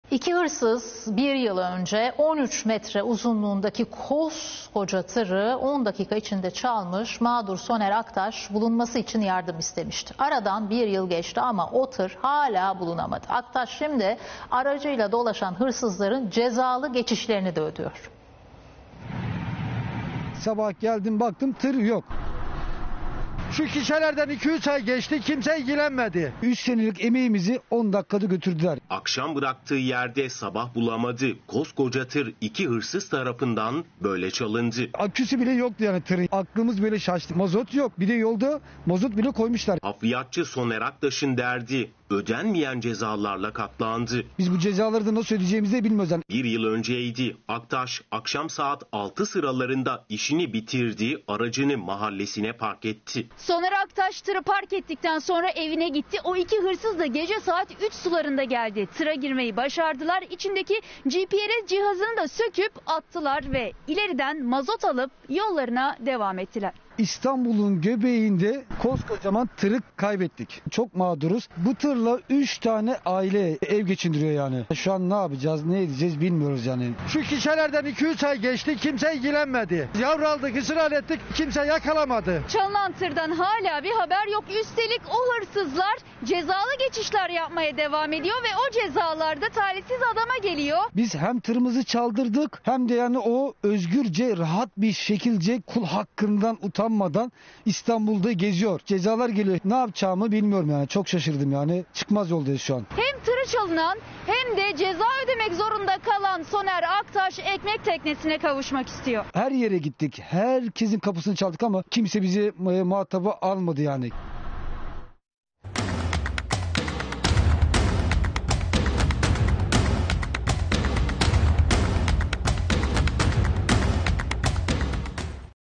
Çalınan Tır'a HGS cezası ödüyor
İKİ HIRSIZ 1 YIL ÖNCE 13 METRE UZUNLUĞUNDAKİ KOSKOCA TIRI 10 DAKİKADA ÇALMIŞ, MAĞDUR SONER AKTAŞ BULUNMASI İÇİN YARDIM İSTEMİŞTİ. ARADAN BİR YIL GEÇTİ AMA O TIR HALA BULUNAMADI. AKTAŞ ŞİMDİ ARACIYLA DOLAŞAN HIRSIZLARIN CEZALI GEÇİŞLERİNİ DE ÖDÜYOR.